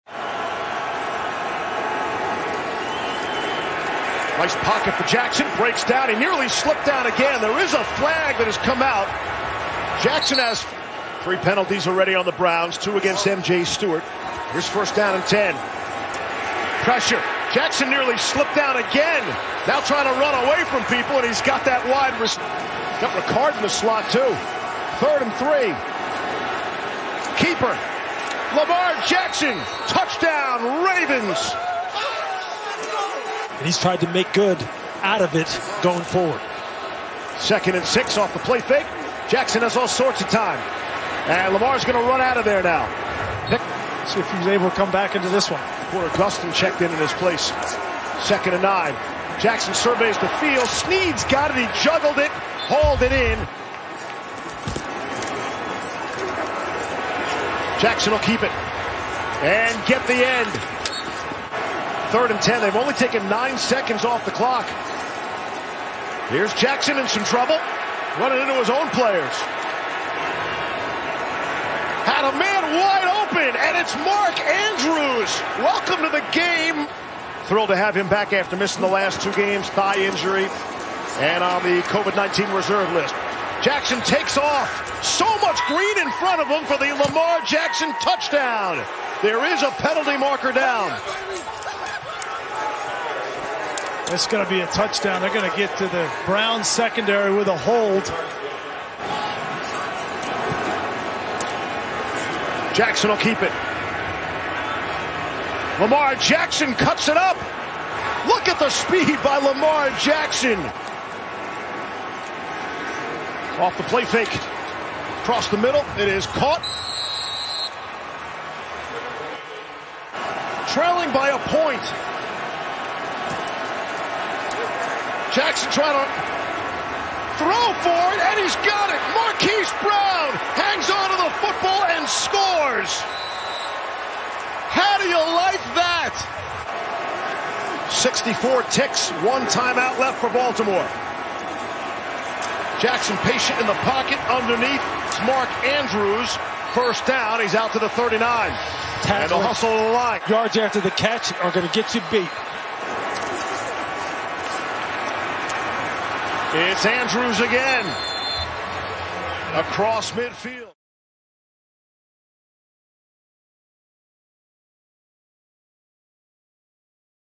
Lamar Jackson Is Michael Vick from Madden 2004 Reincarnated | NFL 2020 Highlights
Lamar could not be stopped tonight! He only threw for 163 yards and one touchdown, but added 124 rushing yards and 2 touchdowns on the ground. The Baltimore Ravens take on the Cleveland Browns during Week 14 of the 2020 NFL season.